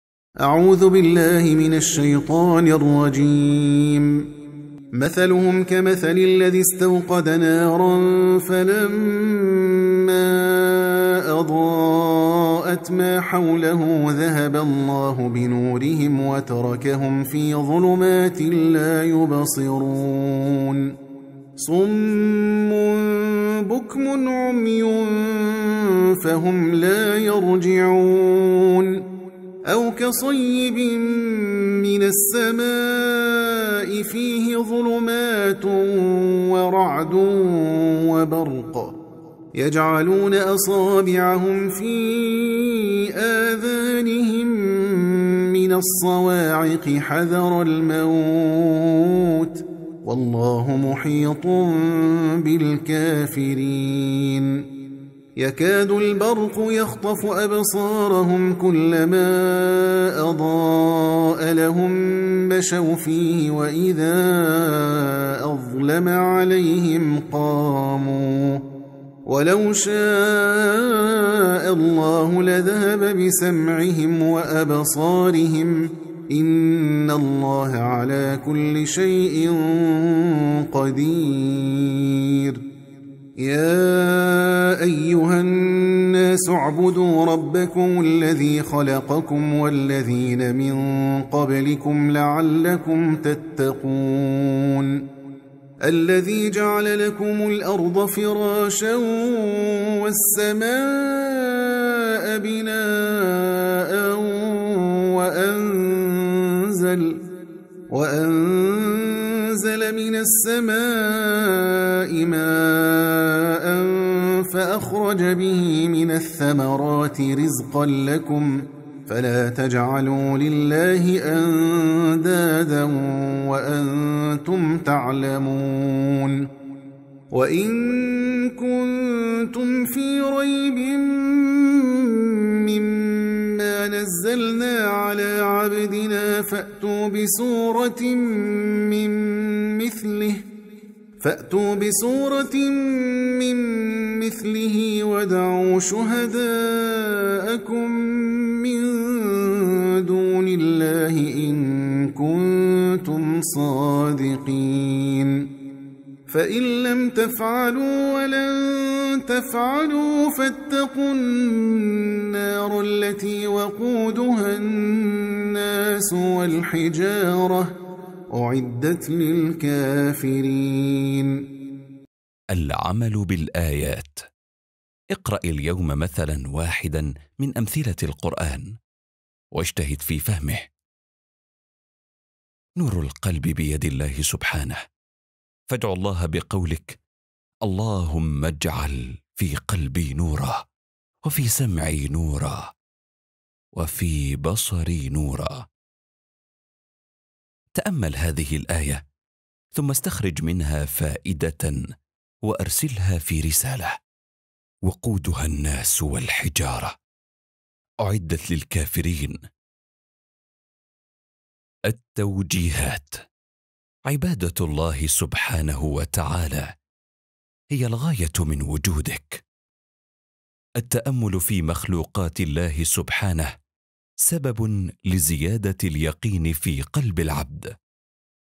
زادك اليوم من كتاب الله آلاف الحسنات بالفهم و العمل و التدبر
الصفحة الثالثة من سورة البقرة
قال ﷺ مَنْ قَرَأَ حَرْفًا مِنْ كِتَابِ اللَّهِ فَلَهُ بِهِ حَسَنَةٌ وَالْحَسَنَةُ بِعَشْرِ أَمْثَالِهَا لاَ أَقُولُ الم حرْفٌ وَلَكِنْ أَلِفٌ حَرْفٌ وَلاَمٌ حَرْفٌ وَمِيمٌ حَرْفٌ